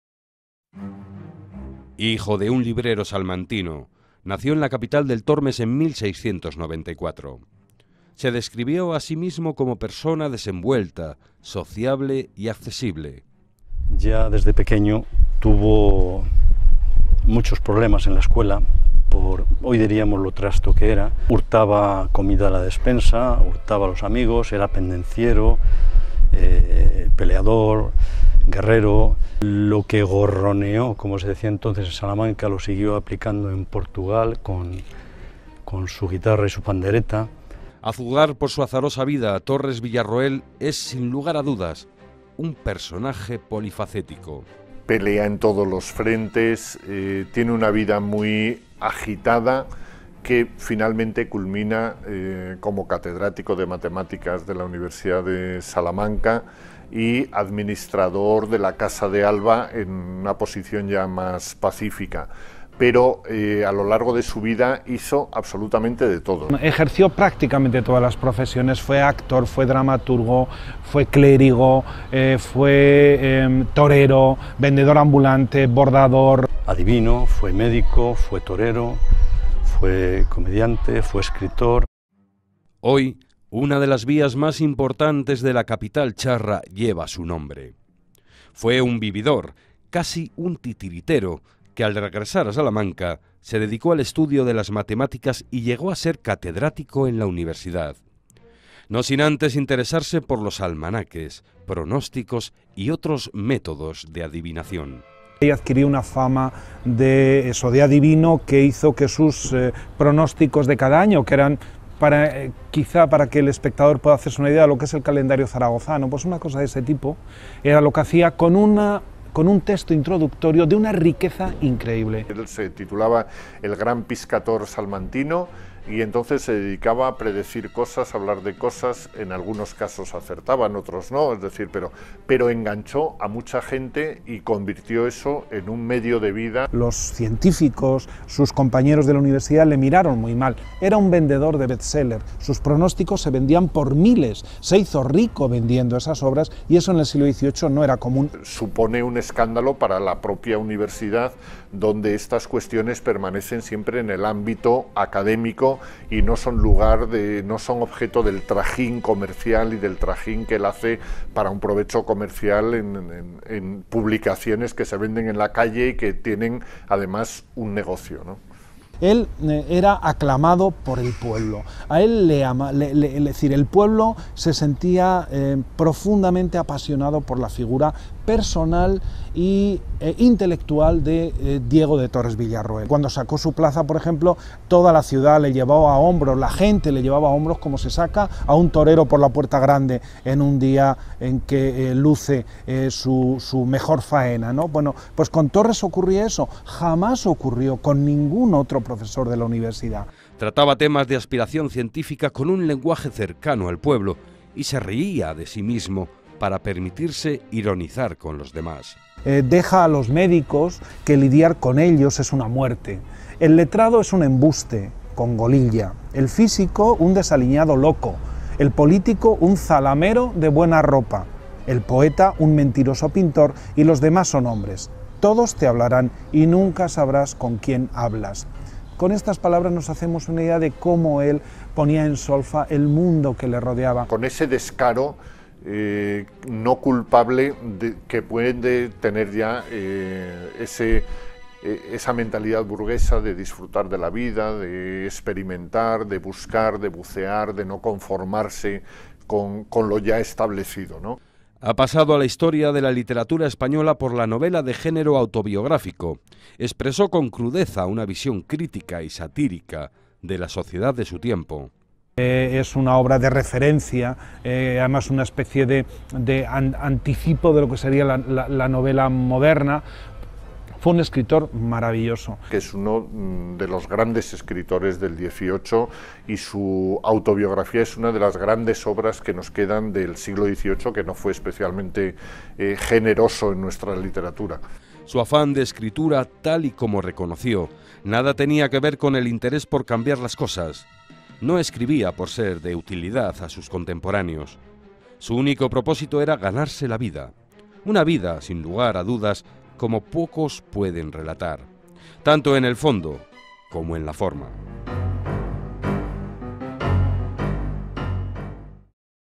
Diego Torres Villarroel
Diego Torres Villarroel Salamanca, 1693 -- Salamanca, 1770

-Escritor, poeta y personaje polifacético, que desarrolló el género autobiográfico como forma novelística.

-Su obra literaria refleja una visión crítica y satírica de la sociedad, algo que adquirió de su admiración por Francisco de Quevedo.

-Obtuvo en 1726 la cátedra de Matemáticas de la Universidad de Salamanca.